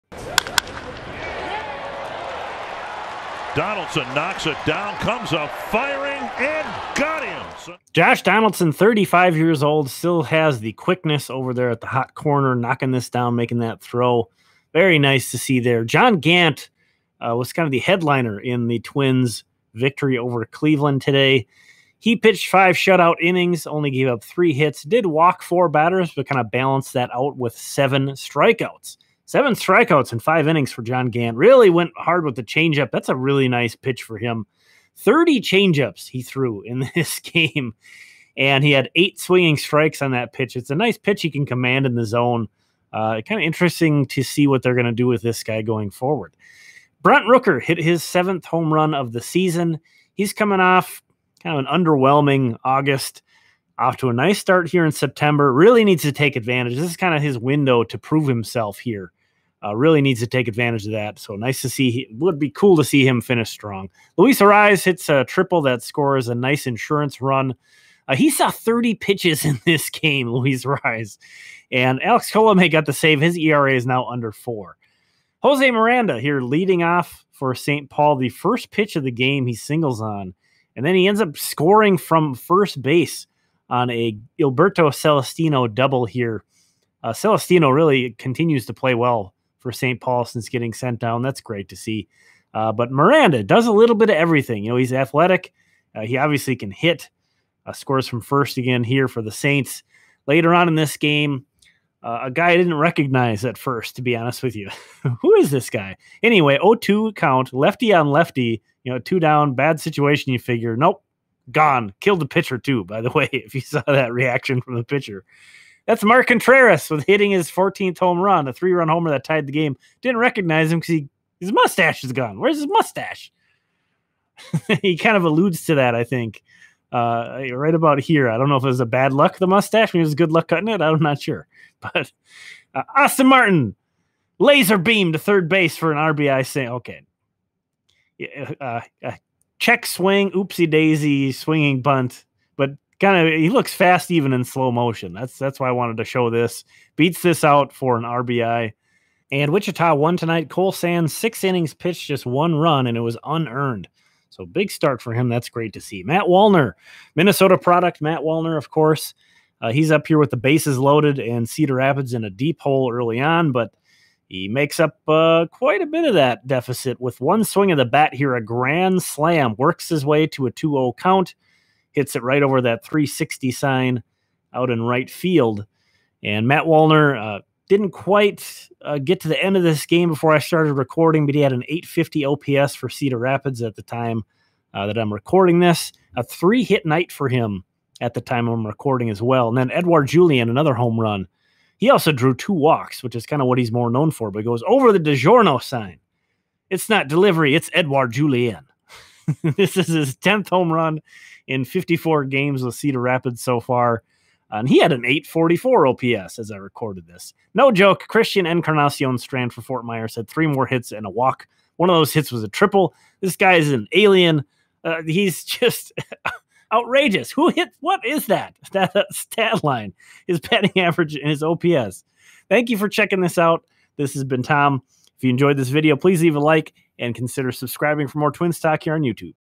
John Gant Goes 5 Scoreless; Matt Wallner Hits Grand Slam For Kernels | Minnesota Twins Recap 9/7/21
Minnesota Twins starting pitcher John Gant delivered five scoreless innings against Cleveland Tuesday night and struck out seven batters. Down on the farm, Matt Wallner was the headliner with a three-hit night that included a grand slam. Also featured in tonight's recap are Josh Donaldson, Brent Rooker, Luis Arraez, Jose Miranda, Gilberto Celestino, Mark Contreras, Austin Martin, Edouard Julien and Christian Encarnacion-Strand.

*Channel formerly known as MN Twins Talk by Tom*